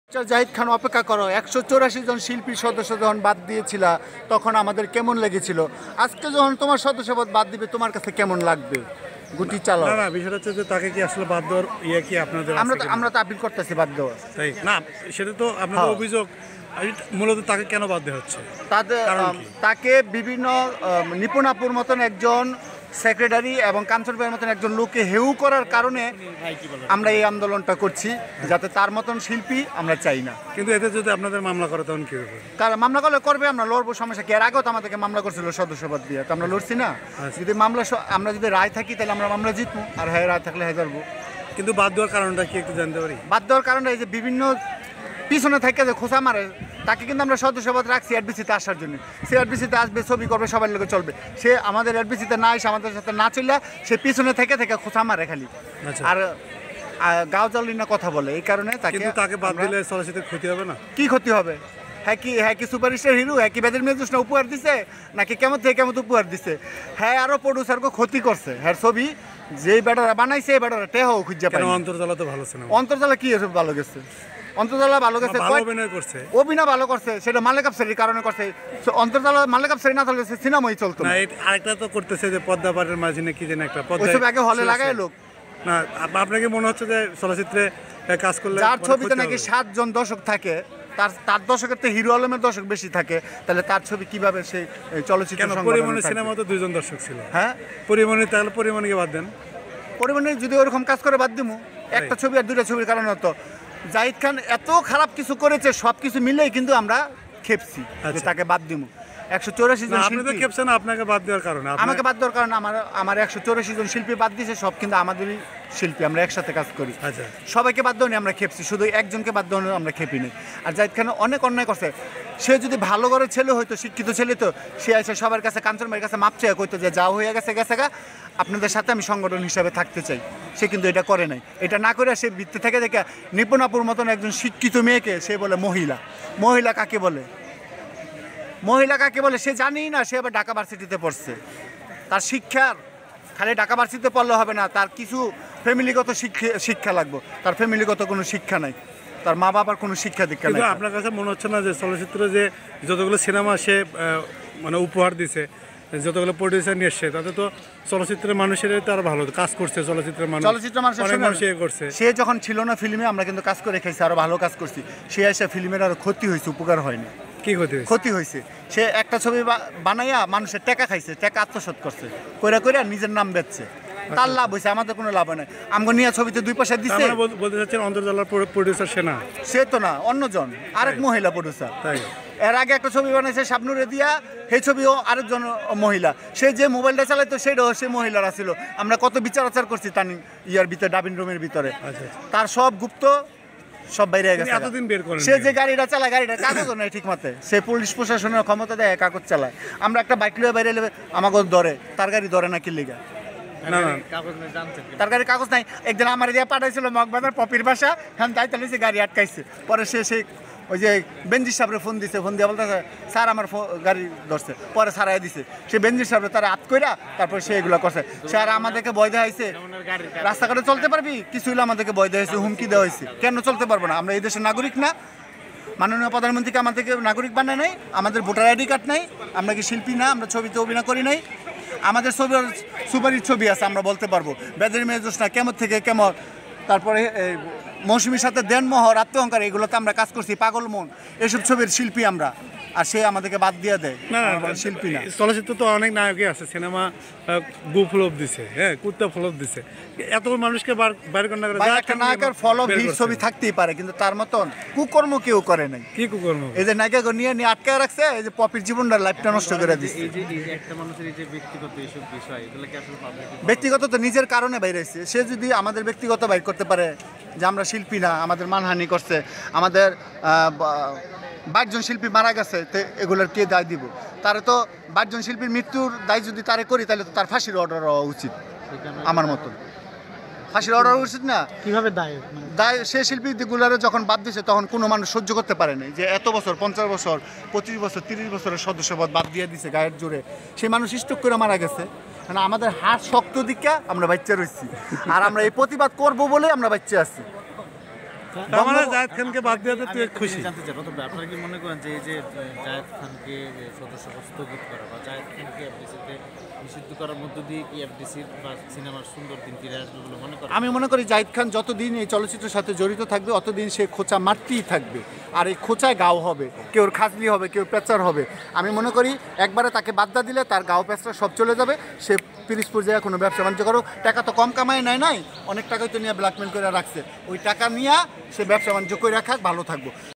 জায়েদ খানের গাড়ীর কাগজ নাই | Roton | Zayed Khan | Fdc
জায়েদ খানের গাড়ীর কাগজ নাই, চলচ্চিত্রে উনি না থাকলে কোন ক্ষতি হবে না - এফডিসির রতন